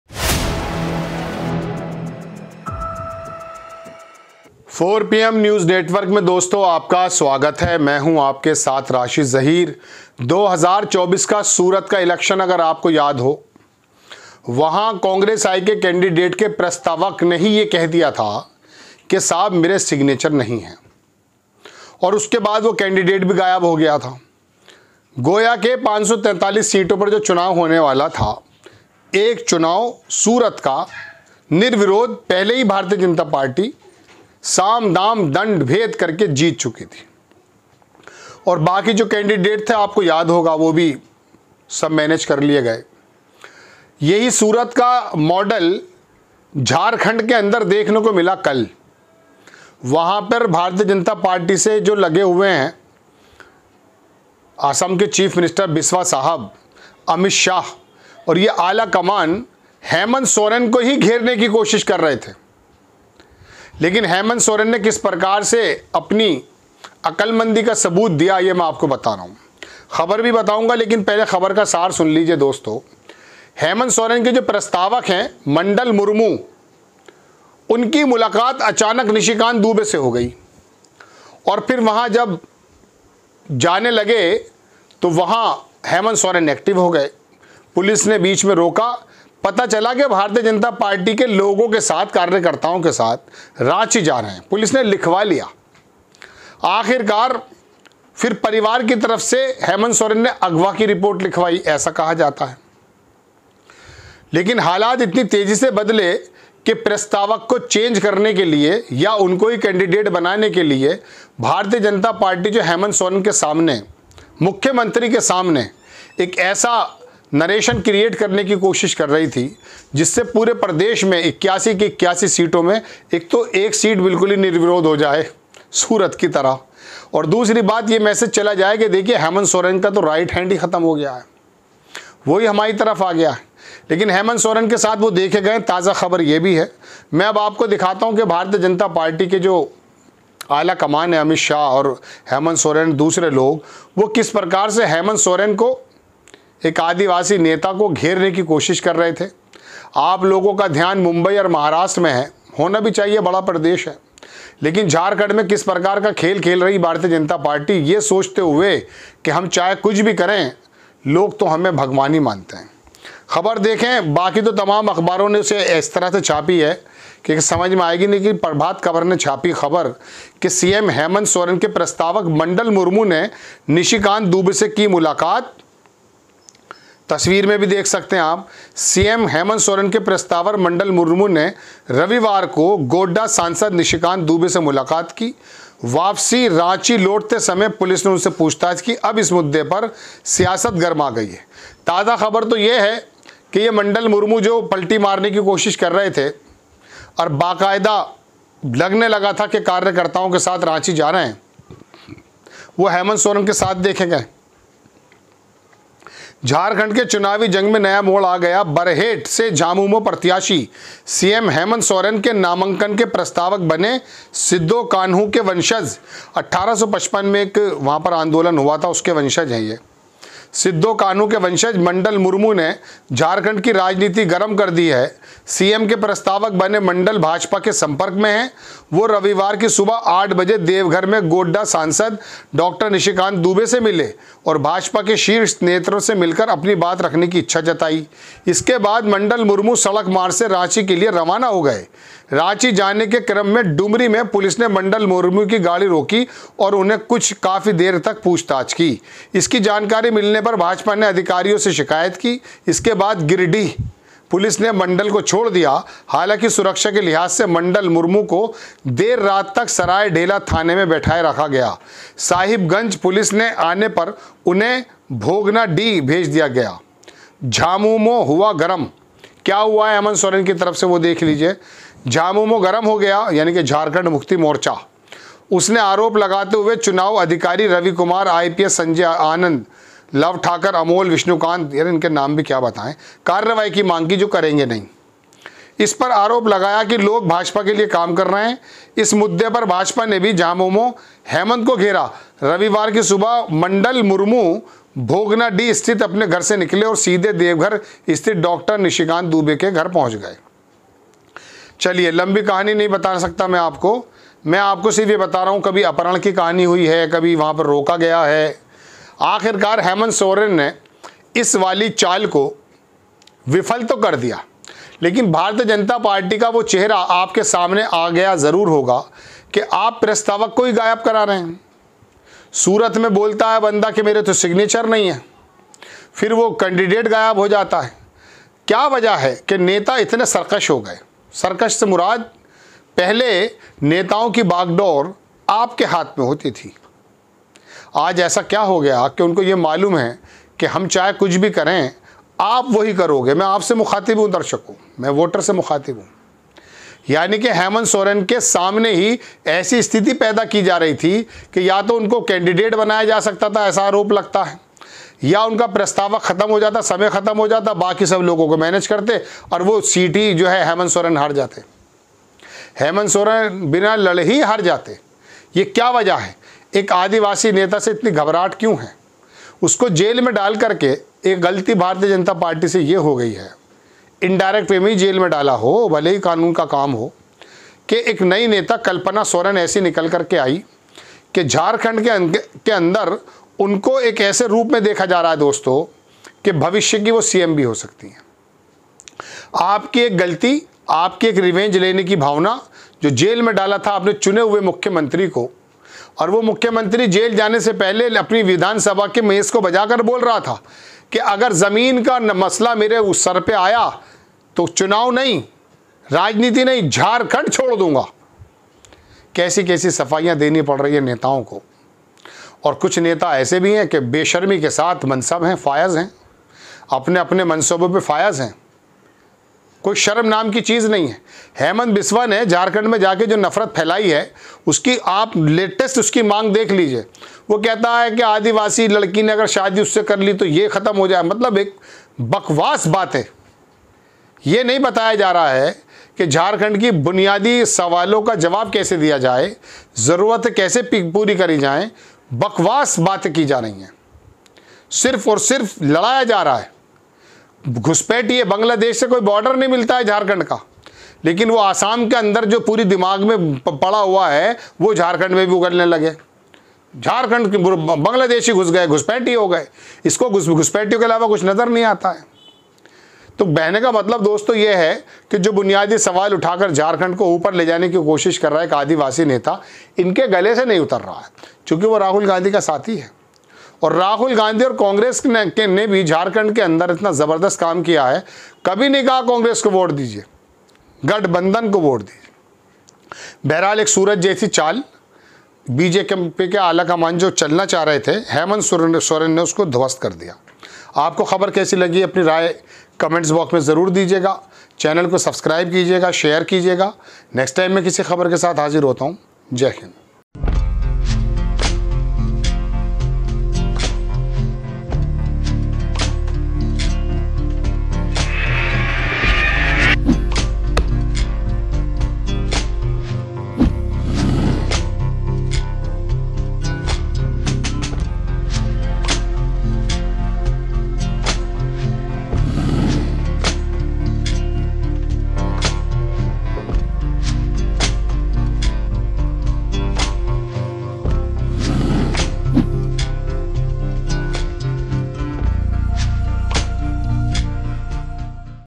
झारखंड में हेमंत सोरेन ने BJP की प्लानिंग कर दी फेल, शाह और बिस्वा पर प्रस्तावक ही गायब करने का आरोप
हेमंत सोरेन का प्रस्तावक मंडल मूर्मू का अपहरण  करने की ख़बरें आती रहीं पता चला कि निशिकांत दुबे से मुलाकात करके BJP ज्वाइन करना चाहते हैं।इस तरह हेमंत सोरेन को बिना चुनाव लड़े ही चुनाव से बाहर करने की BJP अमित शाह बिस्वा की प्लानिंग फेल  हो गई है। हेमंत सोरेन ने पलट दी बाजी